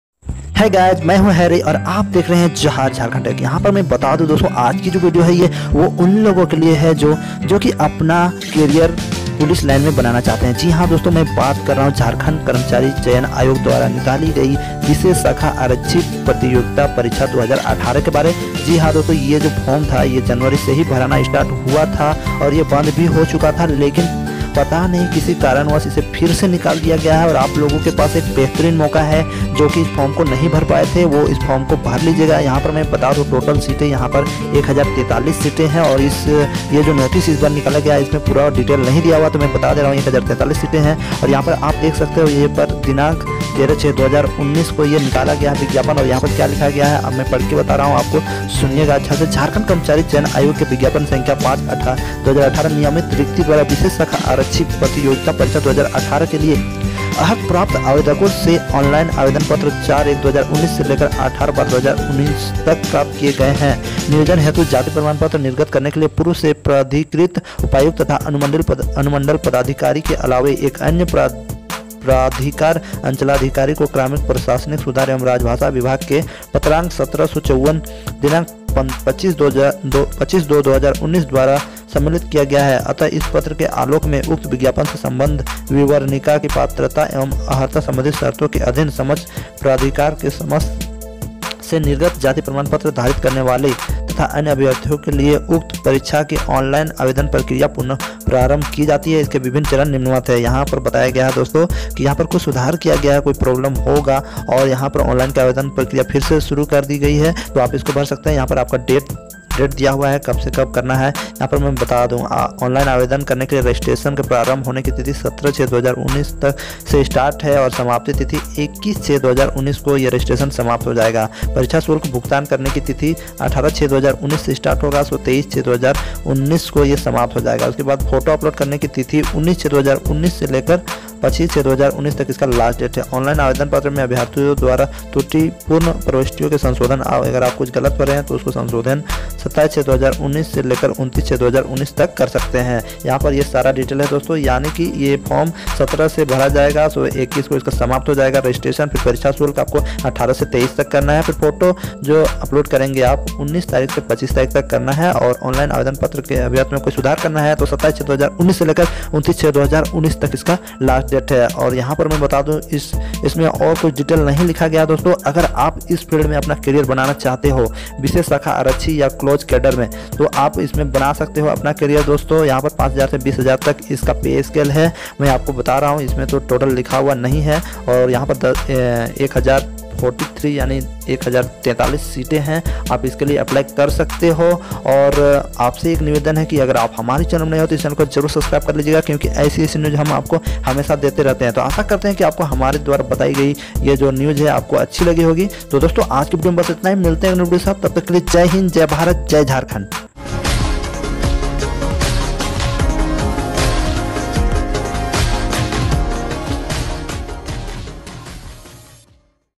JSSC LATEST VACANCY||विशेष शाखा आरक्षी परीक्षा 2019||जल्दी अप्लाई कर दें बहुत बड़ा मौका
age:-

19-25 years for general

19-28 years for obc

19-30 years for st/sc

application fee

st/sc-200 rupees
obc/general-800

official site...